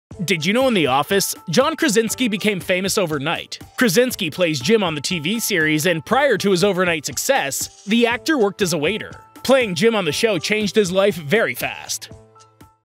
Did You Know In THE OFFICE…
John Krasinski became famous overnight. Krasinski plays Jim on the tv series and prior to his overnight success, the actor worked as a waiter. Playing Jim on the show changed his life very fast.